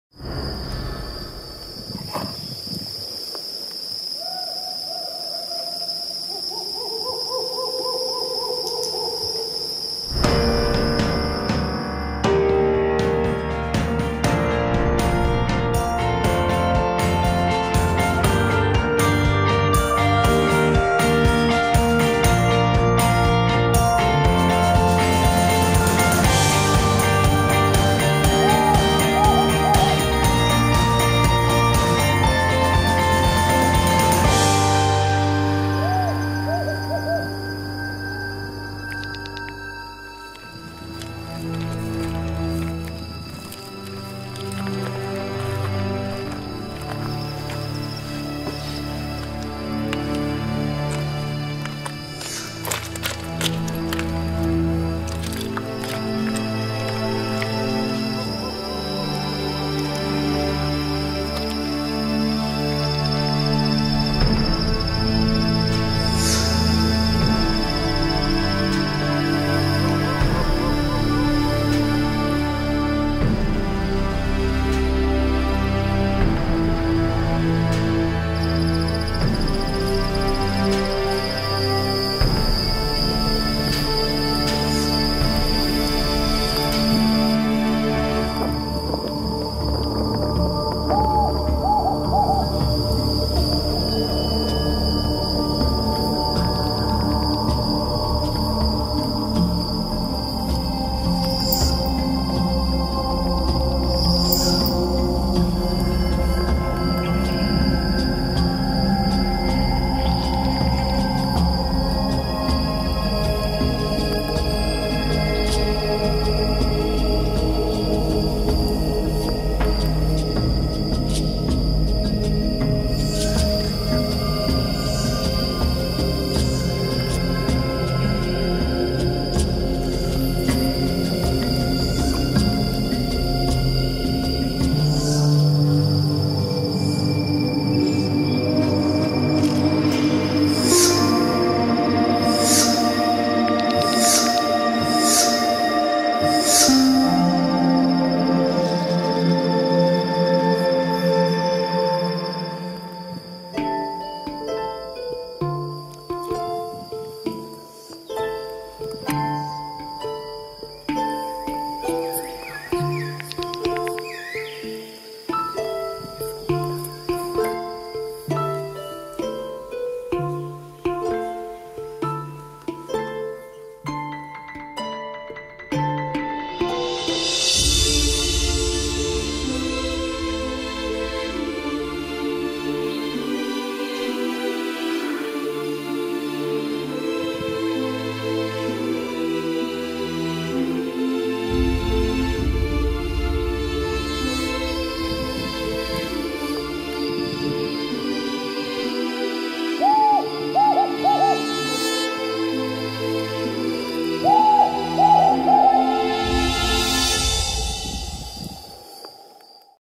לילית המדבר
סרטון שביצענו לו POST PRODUCTION
עבור יובל דקס, רשות הטבע והגנים והחברה להגנת הטבע